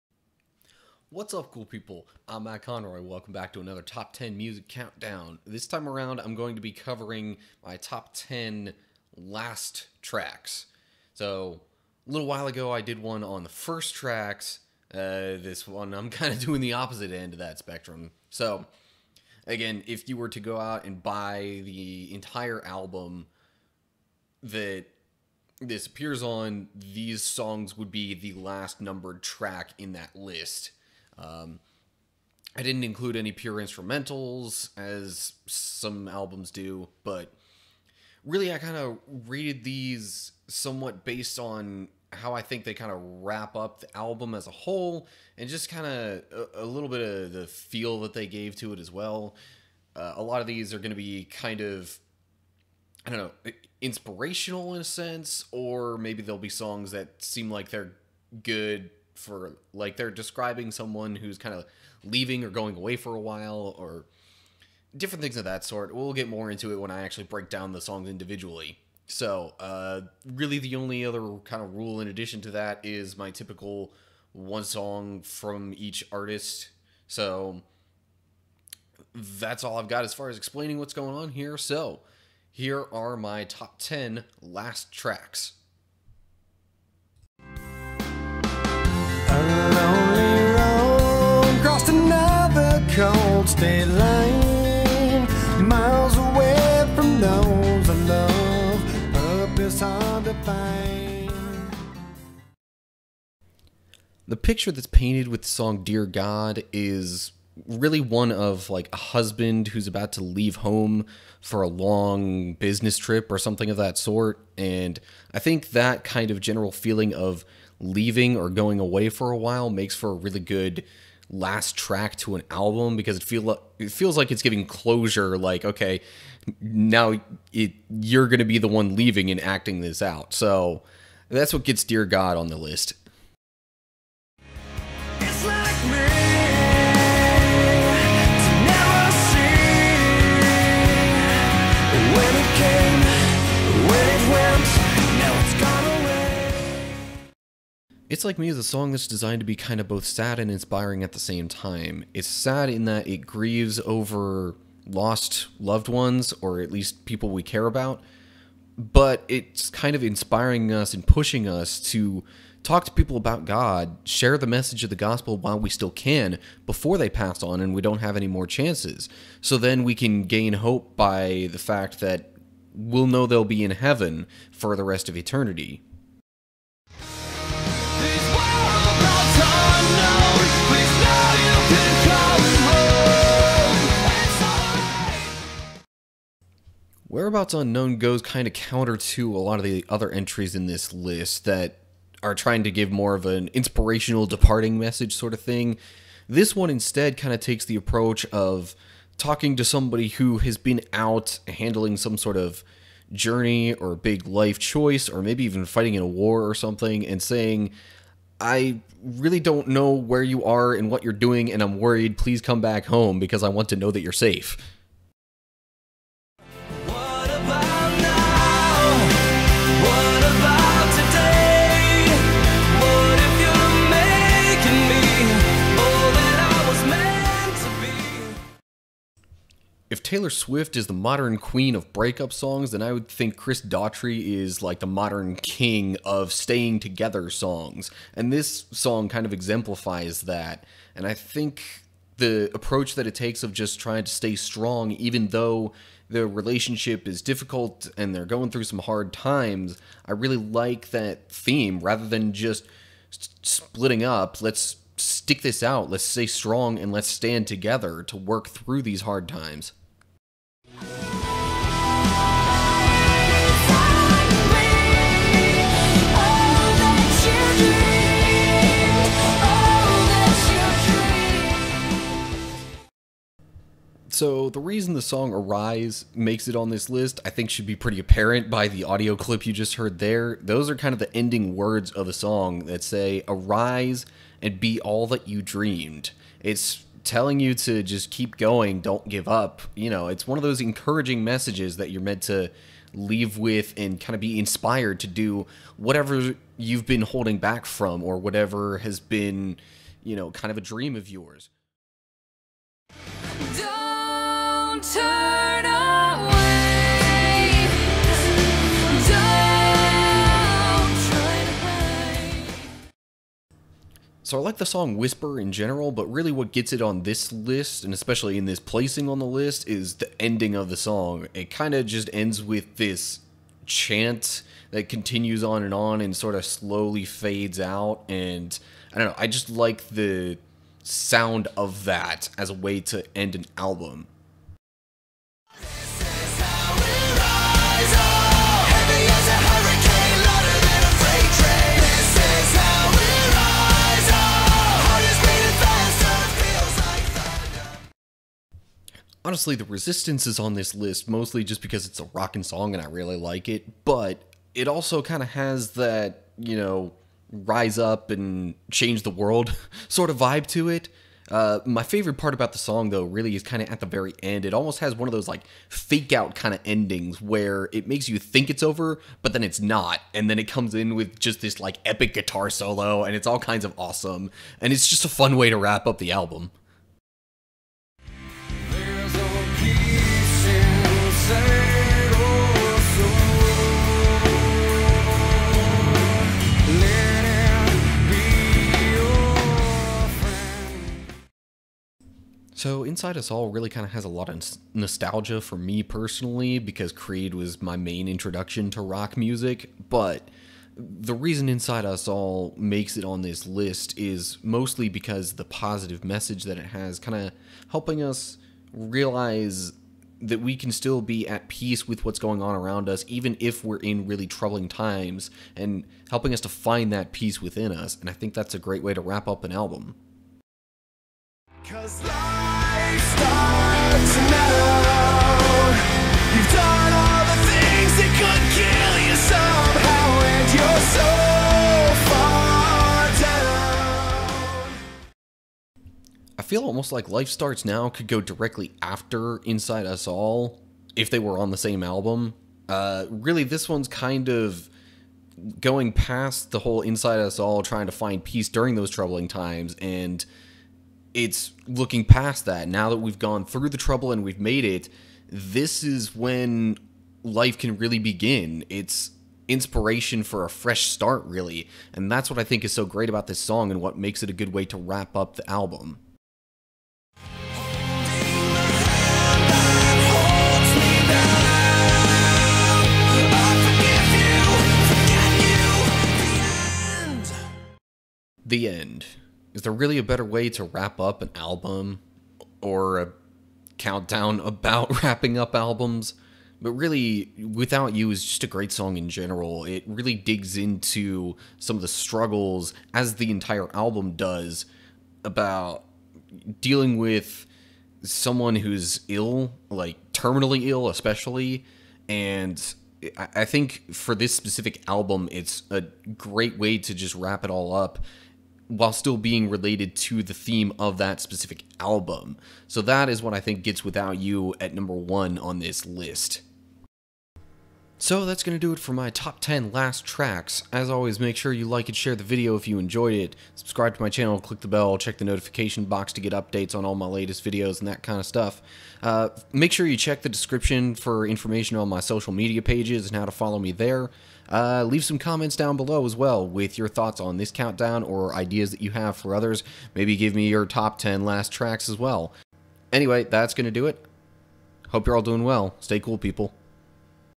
My Top 10 Last Tracks
So we're clear: if you buy an entire album (digital or physical), these songs are the last numbered tracks from their respective albums.  My personal music collection was obviously the greatest limiting factor in this, so let me know what your favorite last tracks are in the comment section below.

Hope you're all doing well!  Stay cool, people!